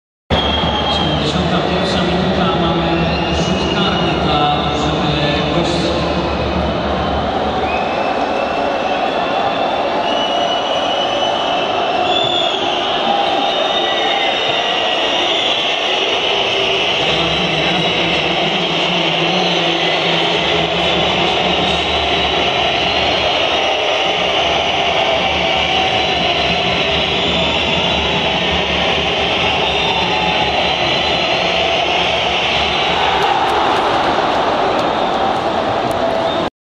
ŚLĄSK WROCŁAW - JAGIELLONIA BIAŁYSTOK 2012-05-03 Tomasz Kupisz bramka (karny) 2:1 HD
Bramka z meczu ŚLĄSK WROCŁAW - JAGIELLONIA BIAŁYSTOK 3 maja 2012 Tomasz Kupisz (karny) 71 minuta.